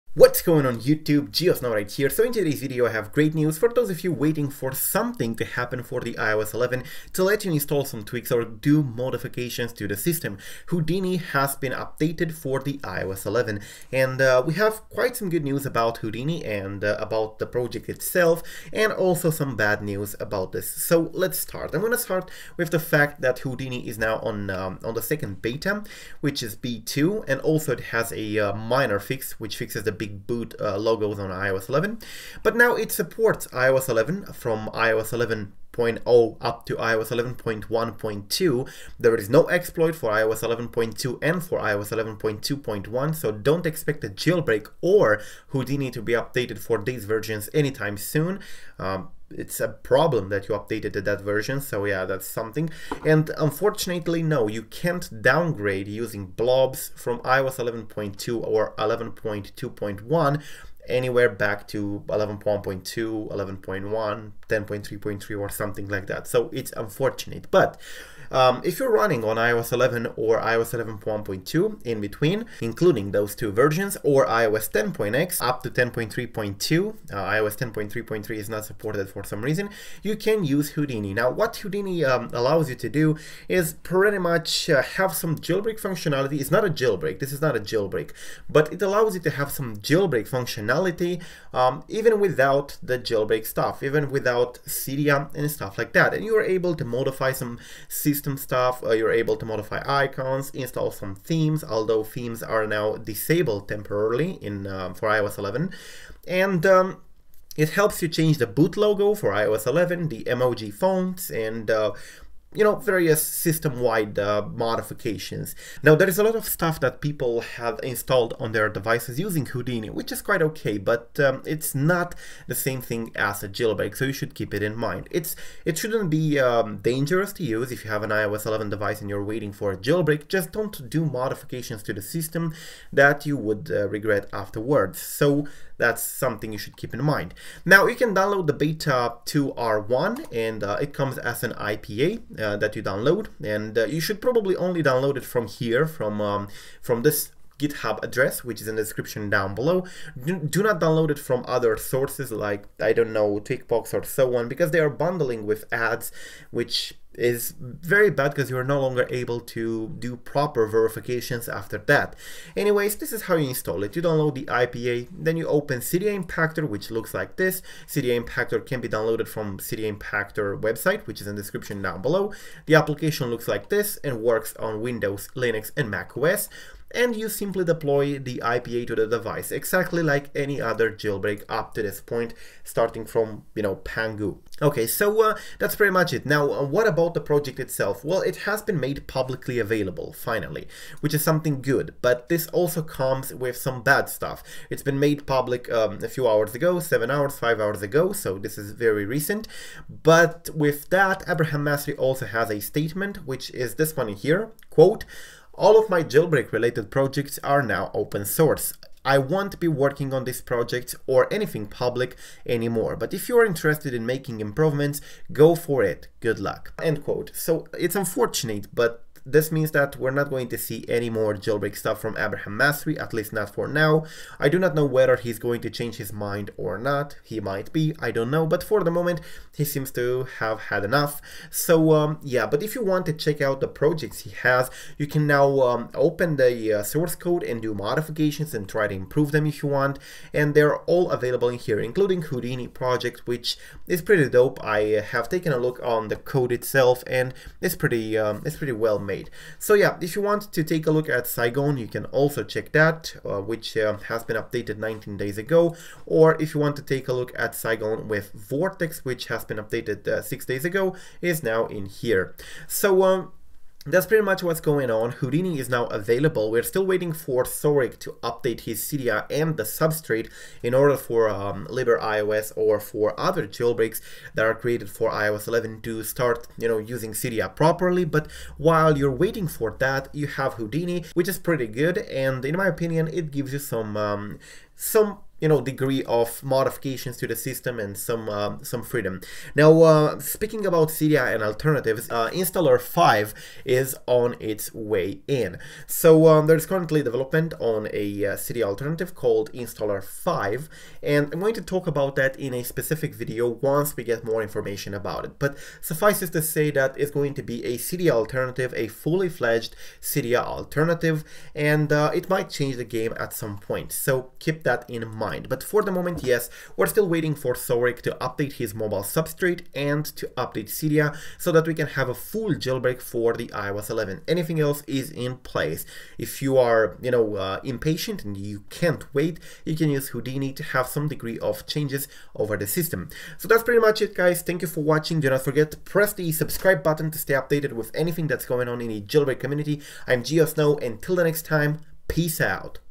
Houdini for iOS 11.1.2/11 RELEASED! Tweak iOS Without Jailbreak
A couple hours ago Abraham Masri has released the source code of Houdini for iOS 10.0 up to iOS 11.1.2 except iOS 10.3.3. Houdini is not a jailbreak, however, it uses jailbreak exploits to allow the user to change system properties, implement custom boot logo, customize the device with Cydia themes (pre-iOS 11), and many other things.

Houdini Beta 2 adds support for iOS 11.0 up to iOS 11.1.2, has a faster startup time, can change icon shapes on iOS 11, blocks ads system-wide (on iOS 11), change emoji fonts (on iOS 11), change boot logo (on iOS 11), add your face to Animoji (on iOS 11 iPhone X only), adds some unnamed safety measurements and the theming is temporarily disabled for iOS 11.

Abraham has followed it up with Beta 2 R1 which fixes the big boot logos on iOS 11.

You should keep in mind that Houdini is now open source, this means that anybody can contribute to it, but it also means that people with bad intentions can add shenanigans to the code and distribute the IPAs, therefore, I recommend getting Houdini only from the official website, trusted sources (in the community), or compile it yourself.

You should not, under any circumstances, install Houdini from 3rd-party app stores, especially those who insert ads in the IPAs. Those are considered untrusted and can become dangerous.

▽ Resources ▽
▶ How to Jailbreak with h3lix Jailbreak: